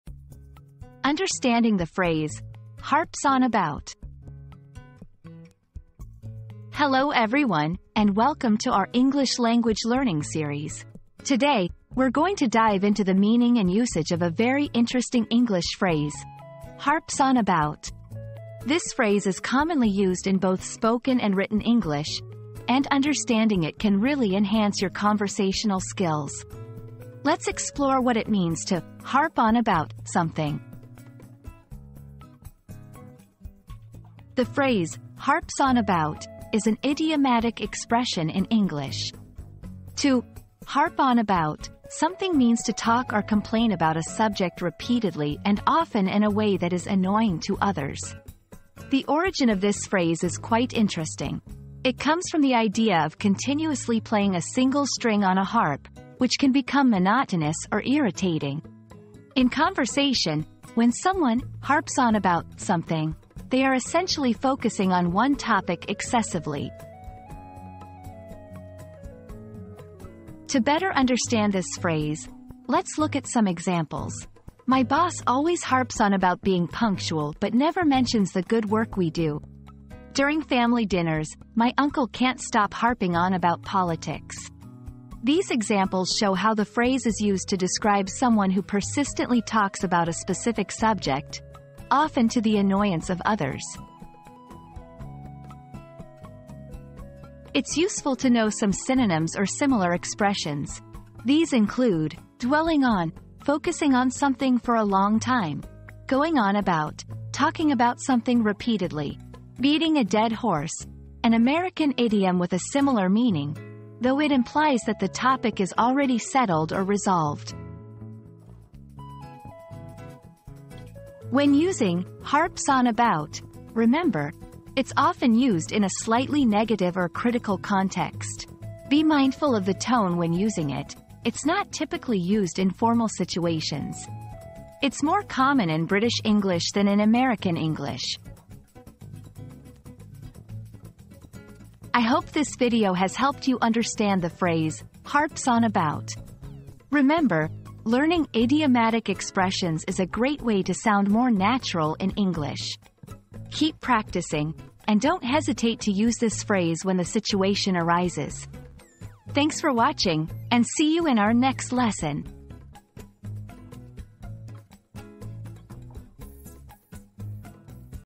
Understanding the Phrase "Harps on About"
Cracking the Code: Decoding the Phrase 'Harps on About' • Unravel the mystery behind the popular phrase 'Harps on About' in this engaging video. Discover its origins, meanings, and usage in different contexts. Expand your vocabulary and gain a deeper understanding of this intriguing expression.

00:00 • Introduction - Understanding the Phrase "Harps on About"
00:33 • The Meaning of "Harps on About"
01:09 • Examples in Context
01:37 • Synonyms and Similar Phrases
02:00 • Usage Tips